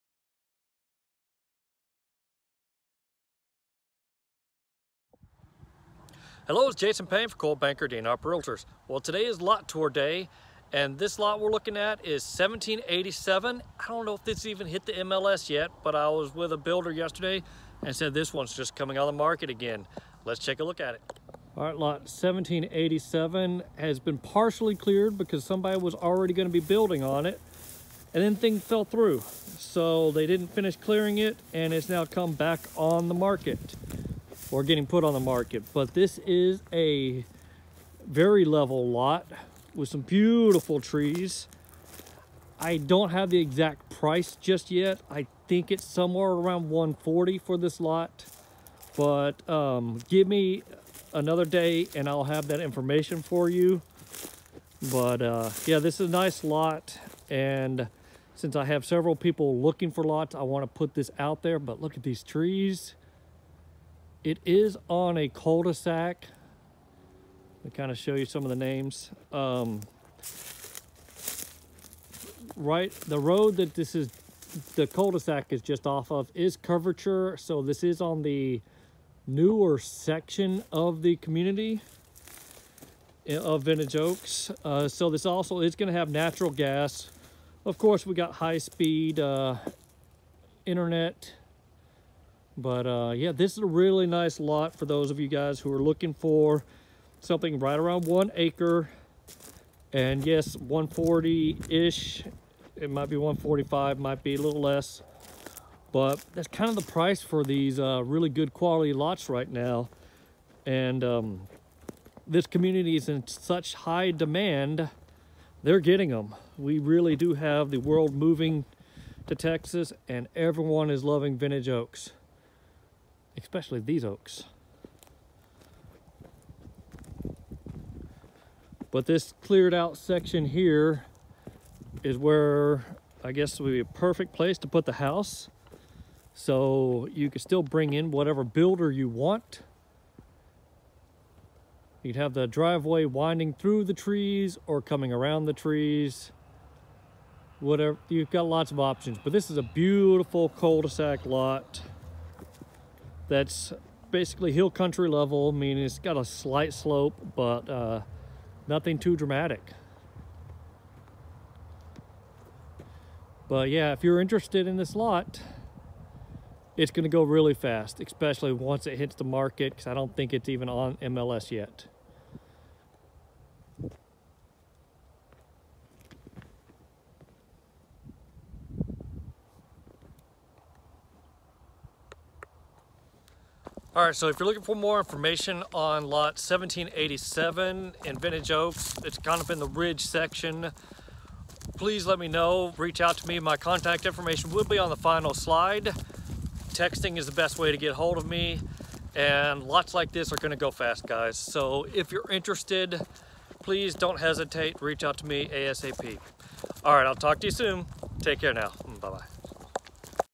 Vintage Oaks Lot 1783 Tour
Vintage Oaks Lot 1783 for sale. Asking price: $139,500. Address 1216 Deerfoot Cir.